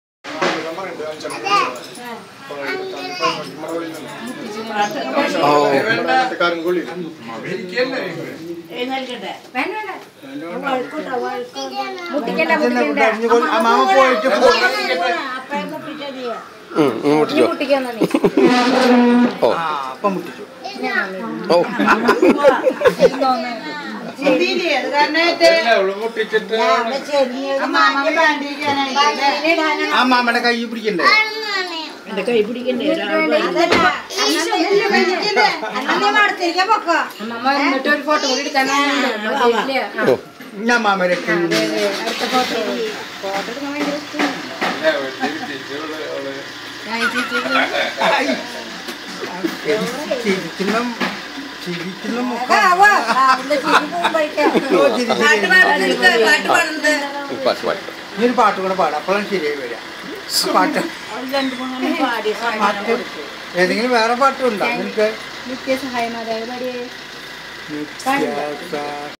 Sr.Honouria in a family gathering at Pazhunana
Sr.Honouria during the visit to the family gathering at Pazhunana.It was a great day for us to spend some time with her after a long gap. She was also very happy to join her kith and kins. Sandy like to play with sr.Honouria.